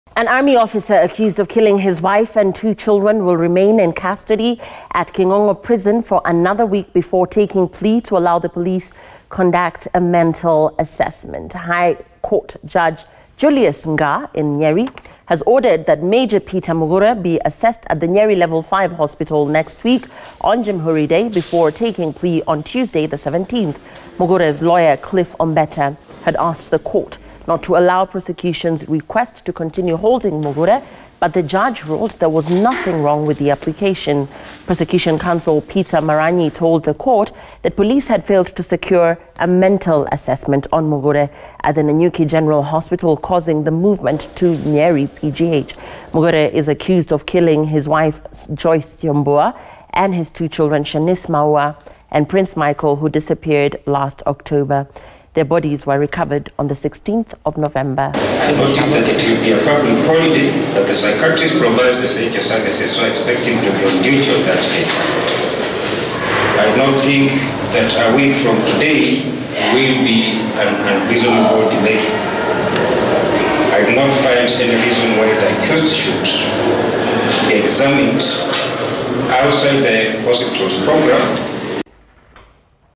Major Mugure to undergo mental assessment before facing charges
An army officer accused of killing his wife and two children will remain in custody at King'ong'o Prison for another week before taking plea to allow the police conduct a mental assessment.